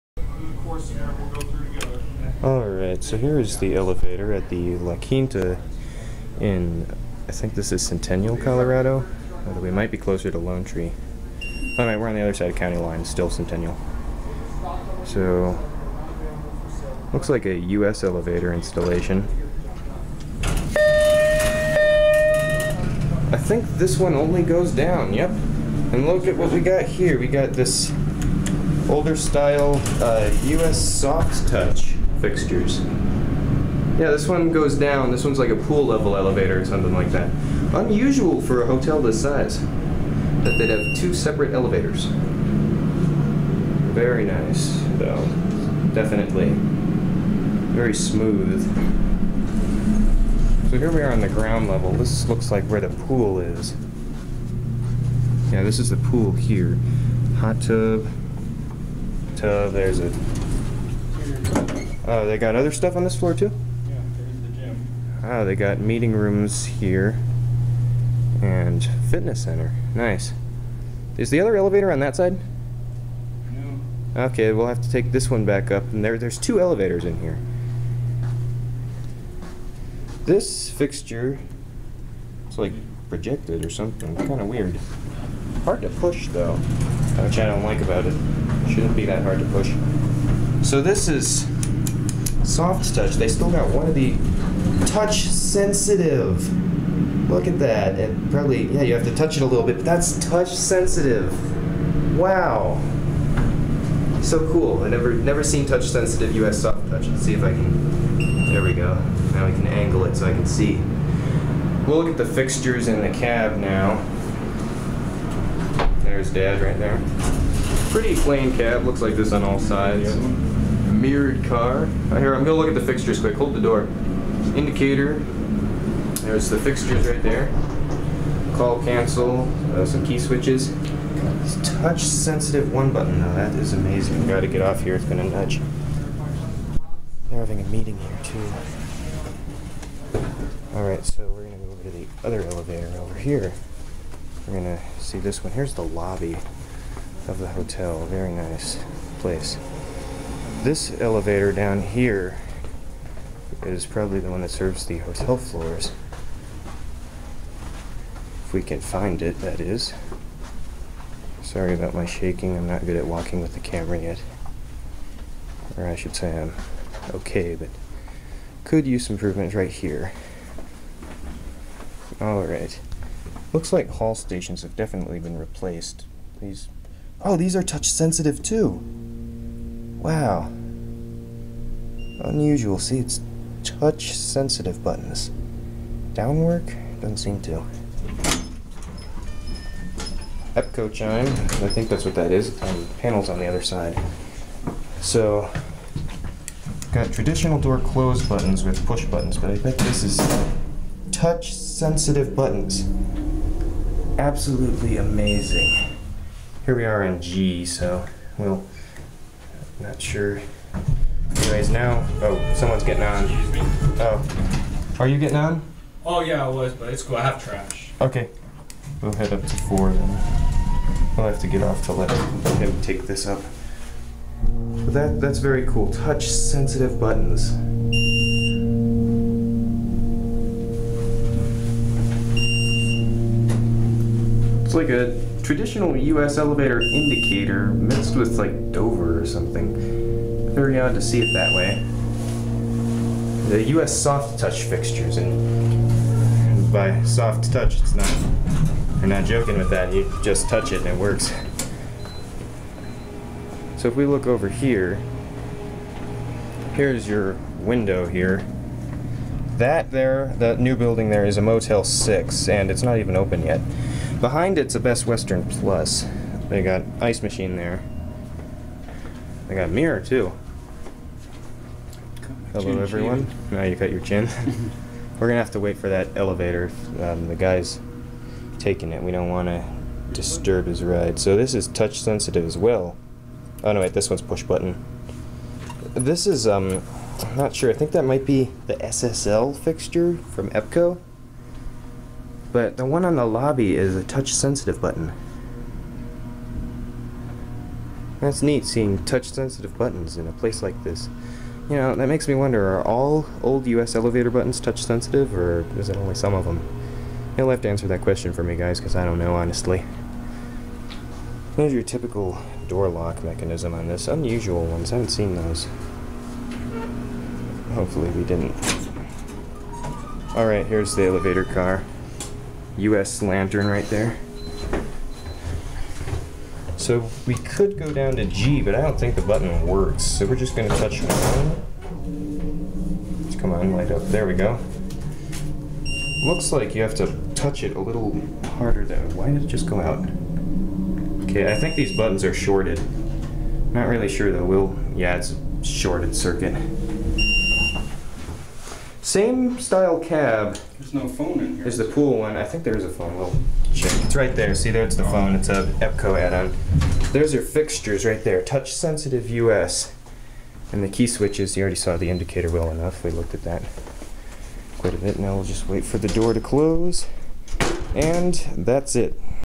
U.S. Hydraulic Elevators at La Quinta Inn, Greenwood Village, CO
This was an interesting hotel. There was an elevator serving only the lobby and pool level and a guest floors elevator, which is unusual for a hotel of this size. Not only that, but the elevators were rare U.S. elevators with TOUCH SENSITIVE Soft-Touch fixtures. Now I understand why they call that fixture line Soft-Touch. Simply lightly touch the button and it activates. The specifications can be found below.

Brand: U.S. Elevator (or possibly (but unlikely) Thyssen)
Motor Type: Hydraulic
Modernized: No
Hall Stations: EPCO WGH
-Illumination Color: Red
-Button Type: Flush, Projecting and Touch-Sensitive
-Panel Finishing: Gold Stainless Steel
Hall Lanterns: U.S. Soft-Touch
-Illumination Color:
  -Up: Green
  -Down: Red
-Lantern Style: U.S. Dot-Matrix Arrows
Cab Stations: U.S. Soft-Touch
-Illumination Color: Red
-Button Type: Touch Sensitive, Projecting
-ADA Compliant: No (Touch Sensitive Not Compliant)
Indicator: U.S. Soft-Touch Dot-Matrix
-Illumination Color: Red
Cab Lanterns: None
Phone: First Elev: Unknown, Second Elev: EPCO
-ADA Compliant: First Elev: Unknown, Second Elev: Yes
-Location of Phone: First Elev: In Cabinet, Second Elev: On Panel
-Safety Sensor Type: Infrared
Capacity: Elev 1: 4000 Lbs, Elev 2: 3000 Lbs
Distance Traveled: Elev 1: 20 Feet, Elev 2: 50 Feet
Floors Served: Elev 1: *G, 1. Elev 2: *G, 1-4
Year Installed: 1989 or earlier
Other Observations or Details: These elevators are, what I would say, was an attempt at luxury elevators.